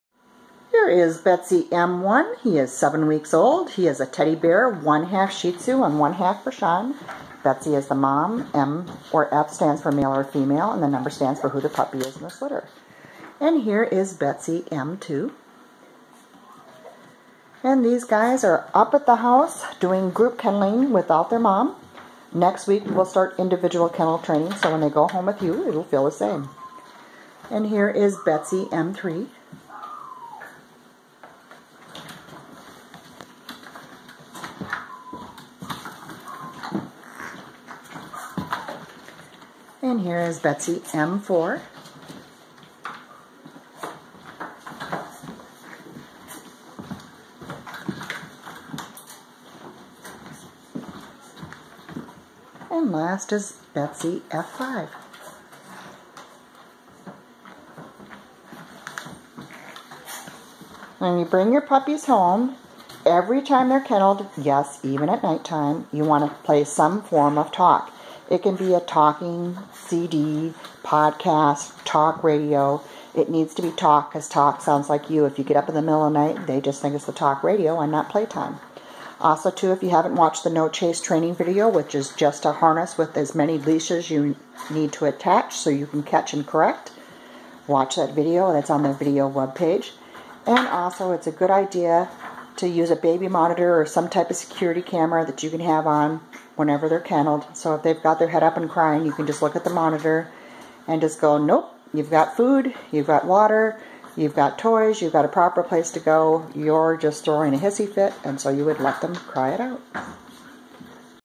betsee02212019m1m2m3m4f5 on 4 13 2019
1/2 shih tzu & 1/2 bichon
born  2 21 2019
0 available $616 male
betsee m1 m2 m3 m4 are SOLD
0 available $831 female
betsee f5 is SOLD
m=male f=female
mother 7# father 11#
range 7-13#
7 weeks on 4/13
8 weeks on 4/20
***READY 9 weeks pickup 4/27
non-shed hypo-allergic
7  weeks